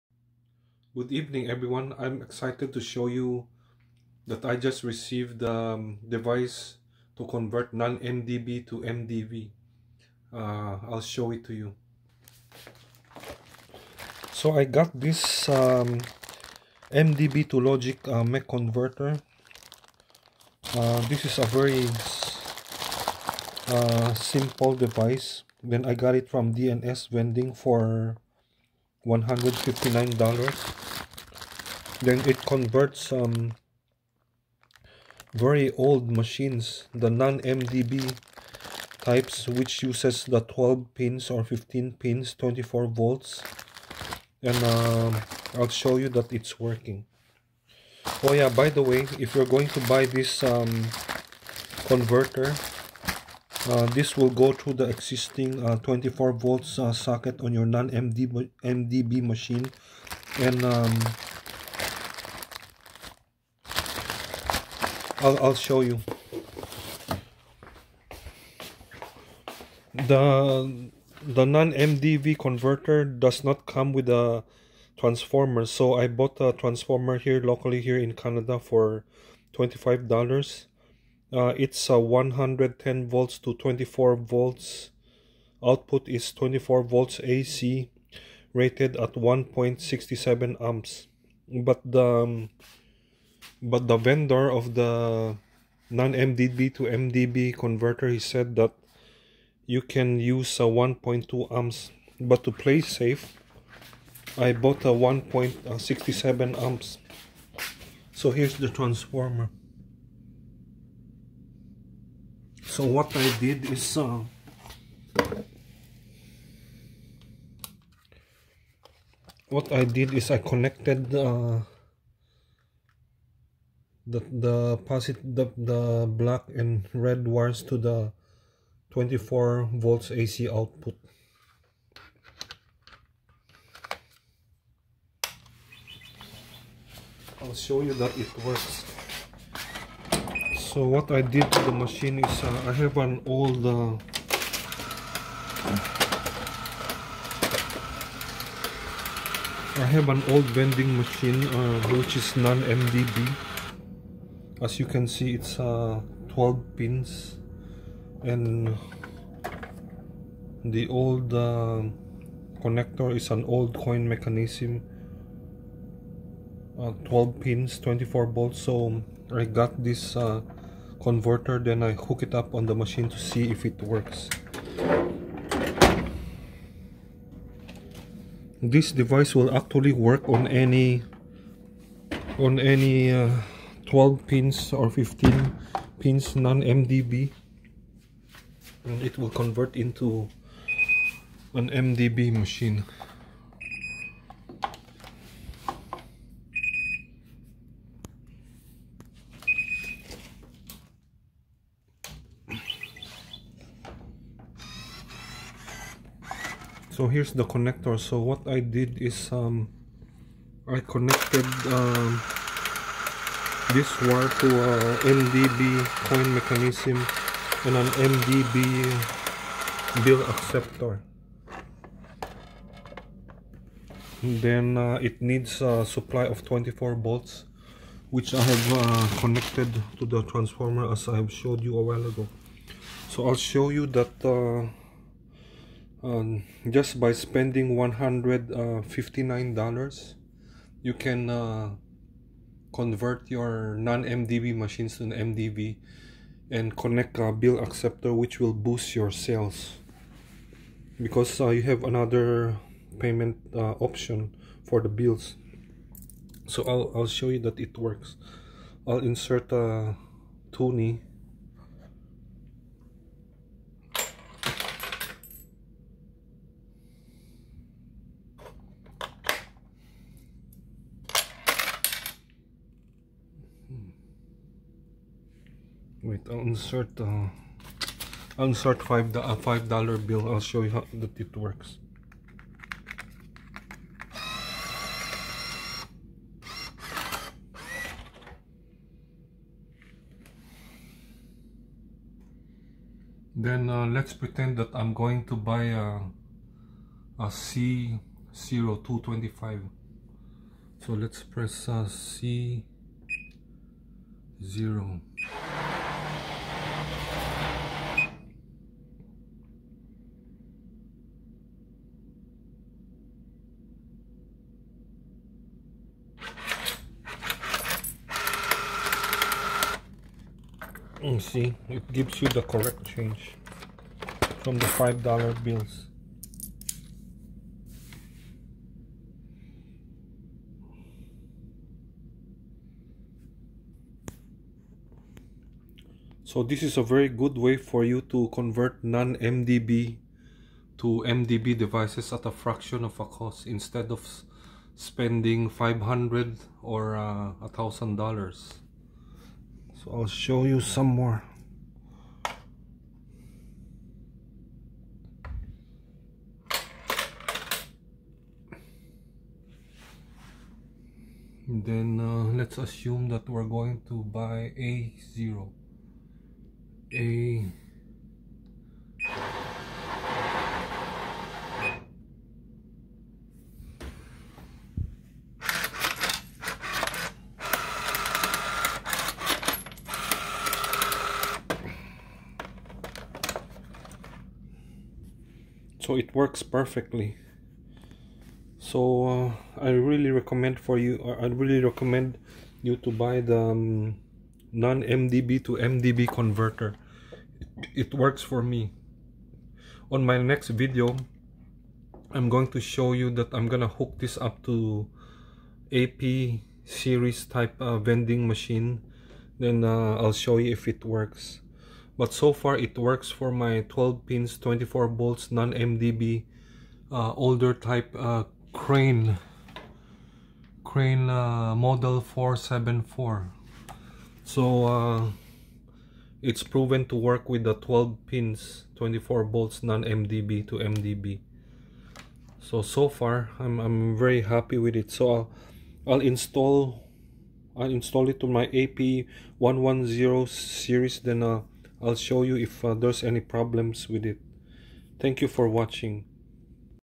Convert old vending machine 12 PINS non mdb to mdb that can run bill acceptor and debit card reader
I tested this on an old Crane GPL 474 Non MDB Combo Vending Machine using 12 PINS 24 Volts Connected to the Non MDB Coin Mech.
Here is a Step By Step:
I switched off the power of the Crane GPL 474 Non MDB Vending Machine
Took out the 12 PINS Connection of Non MDB Coin Mechanism
Installed the MDB Converter 12 PINS to the existing 12 PINS Connector on the Crane GPL 474
Installed the MDB Converter Kit Red and Black Wires to a Transformer ( Input : 110V Output : 24VAC 1.6Amps )
Connected the MDB Converter to a Coinco 6 Tube Coin Mech and Coinco Bill Acceptor. ( You can also use MEI Bill Acceptor )
Switch On the Power to the Vending Machine
Switch On the Power of the Transformer ( Input : 110V Output : 24VAC )
The whole thing works fine at my garage

I will install this on site by tomorrow on an AP 110 Series Vending Machine and will again make a video to help you out on exactly how to do it by yourself.

This saved me a ton of money and this will also save your older Non MDB vending machines and update or upgrade to accept Bills which will definitely boost your sales!

In this Pandemic times, it is essential for you to make use of all your spare parts such as MDB Bill Acceptor, MDB Coin Mechanism and Purchase a NON MDB to MDB Converter!

I will try to connect a Payrange, Nayax Reader or USA Technologies ePort Reader and will make a video as well in the future to see if they will all work in synergy.